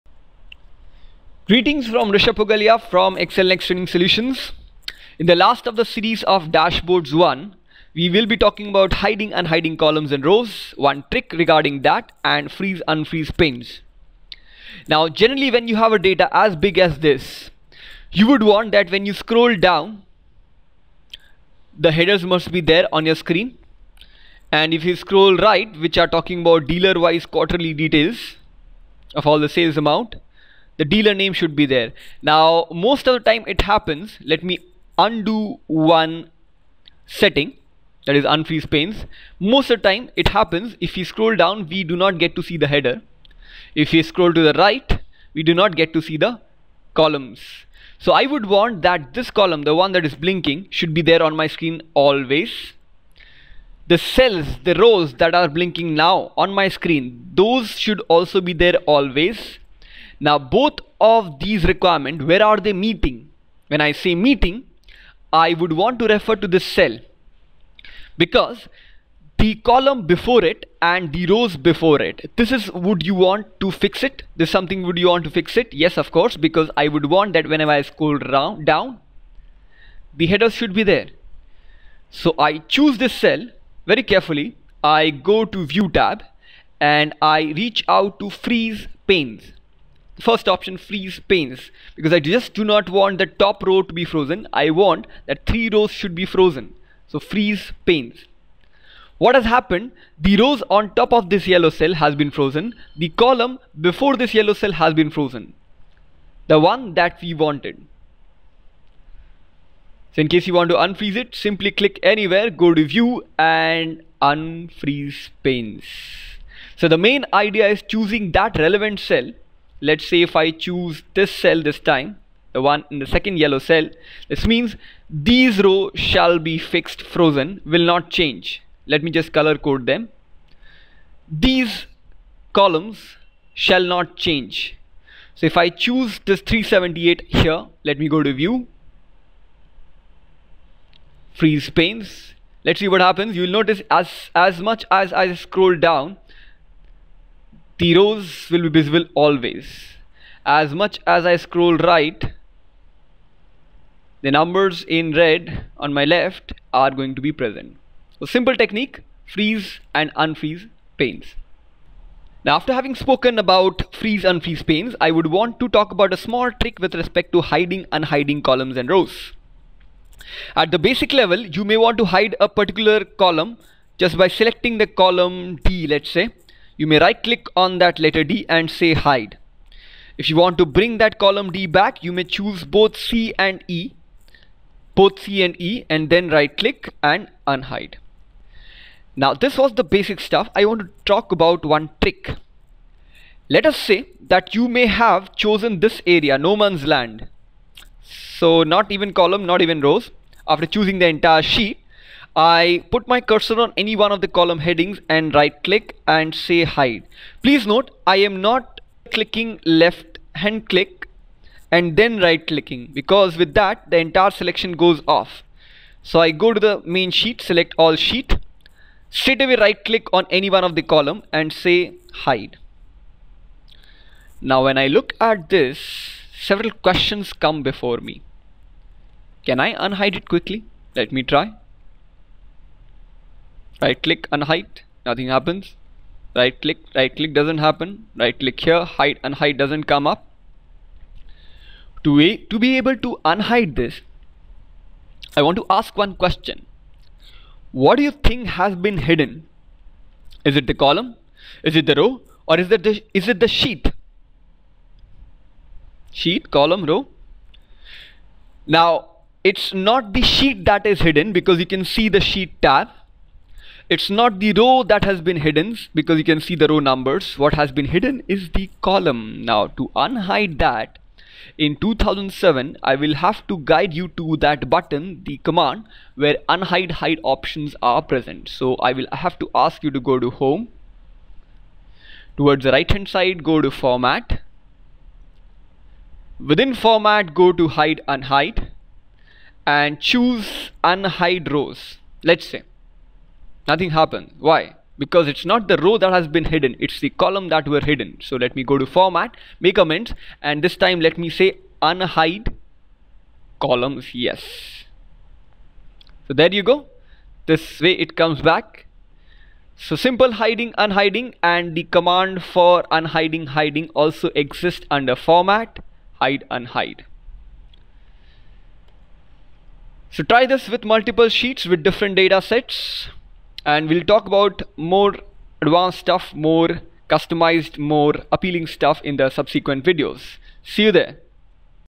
Excel Freeze & Unfreeze Panes (Bonus: Hide & Unhide Rows or Columns)
In this tutorial, you will learn two amazing tricks in Excel which will blow your mind.
1) How to Freeze & Unfreeze Panes in Excel
2) How to Hide and or Unhide Columns & Rows in Excel

We Covered the following topics:
00:07 Agenda of the video - Hiding & Un-hiding columns & rows
00:12 Agenda of the video - Freeze & Unfreeze Panes
00:18 Example of Freeze Rows in Excel
00:24 Example of Freeze Columns in Excel
00:48 Example of Freeze Panes in Excel
01:25 How to Freeze Panes in Excel
01:50 How to Unfreeze Panes in Excel
01:59 Howe to freeze multiple rows & columns in Excel
02:50 How to hide one column in Excel
03:00 How to un-hide one column in Excel
03:10 How to hide multiple columns in Excel
04:29 How to unhide multiple columns in Excel